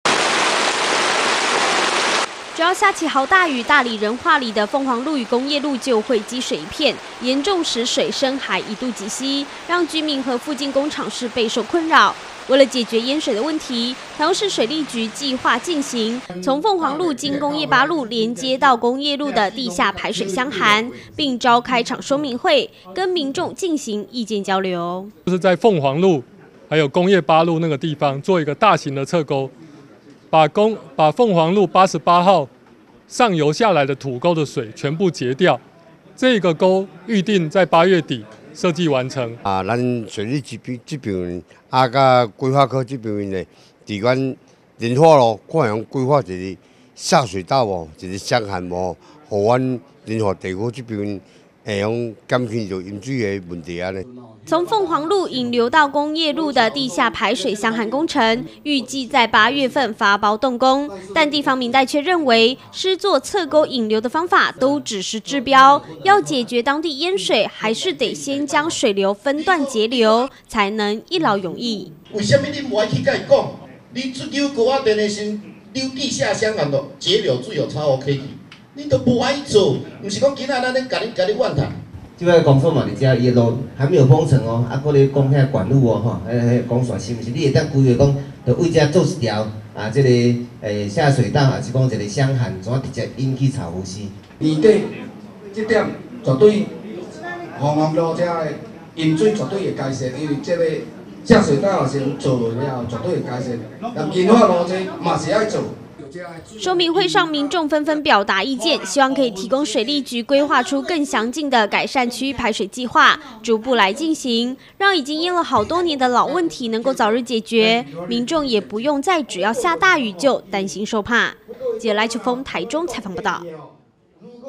大台中新聞-鳳凰仁化路淹水改善說明會
大里仁化里一帶由於地勢低窪，加上排水設計不良，每逢大雨包括鳳凰路與仁化路就容易造成積水，不只居民無奈，店家無法做生意更是苦不堪言，在地方民代爭取下，台中市水利局預計從鳳凰路，經工業八路，連接到工業路的地下排水箱涵，為此水利局特別召開一場說明會，跟民眾進行意見交流。「以上新聞可從哈TV APP收視」。